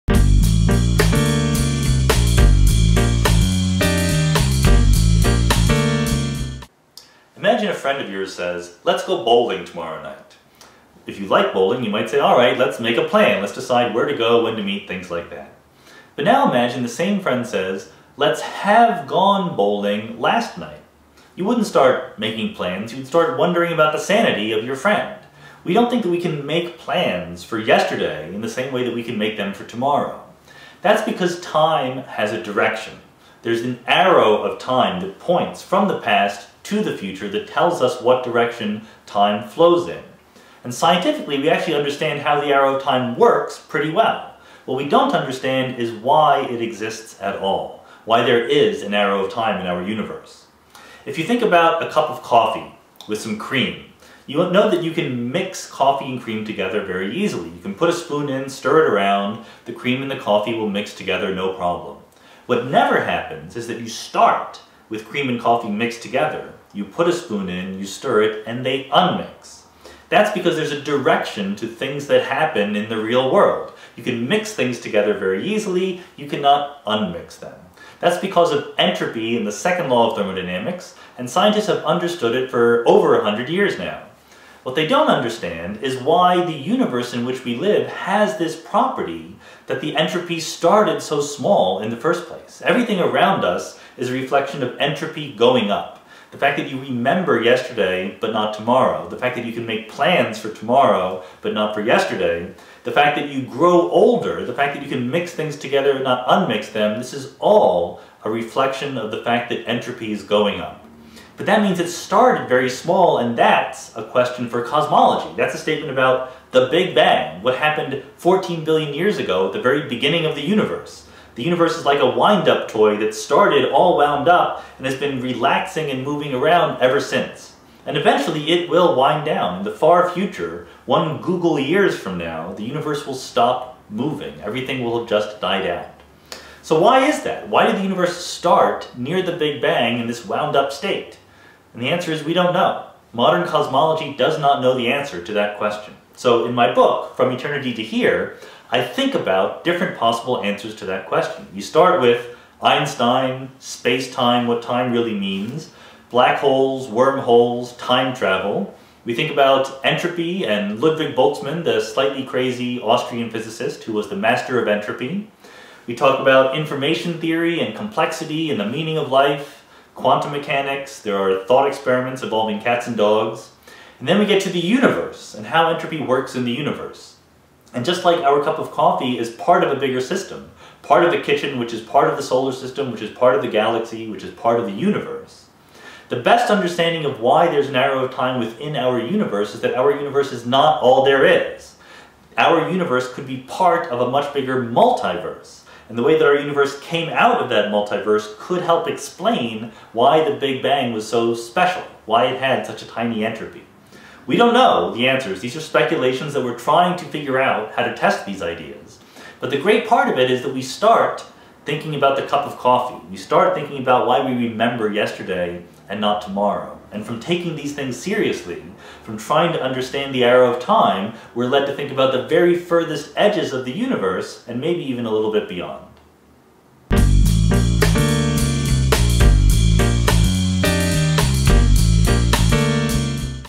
From Eternity to Here: The Quest for the Ultimate Theory of Time
Sean Carroll's book about the origin of the universe and the arrow of time.